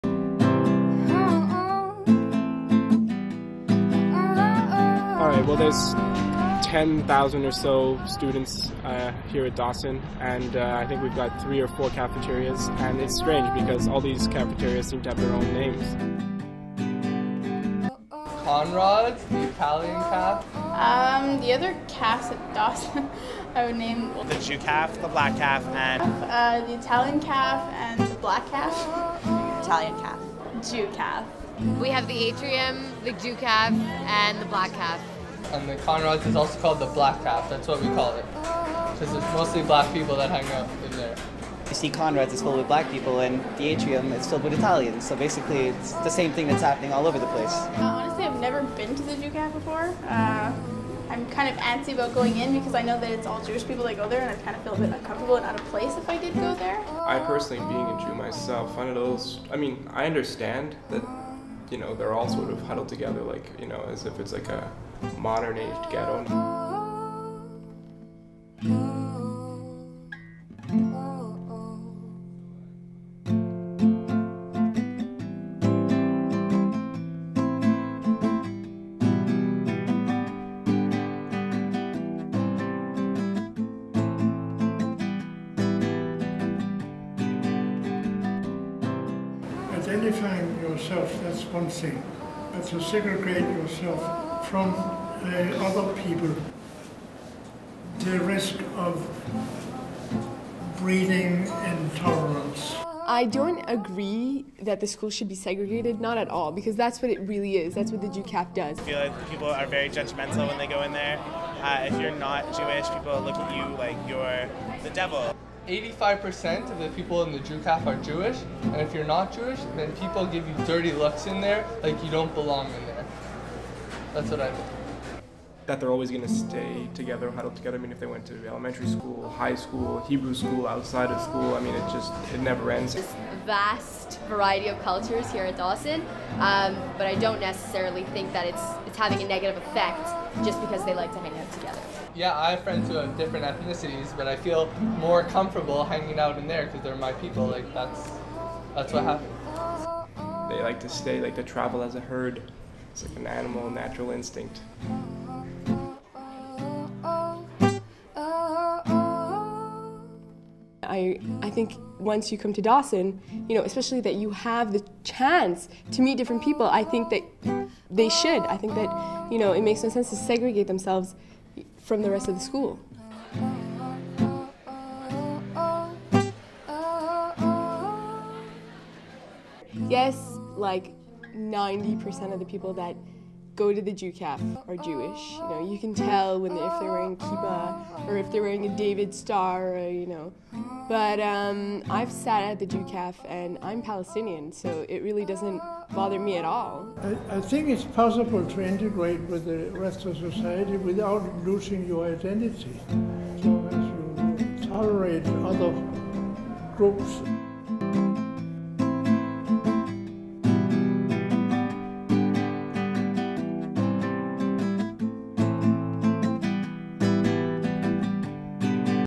Generation Segregation
A short documentary about the segregation at Dawson College by Emilie Johnsson, Michael Bitton, Taylor King and Kara Mercado made in the Fall of 2009.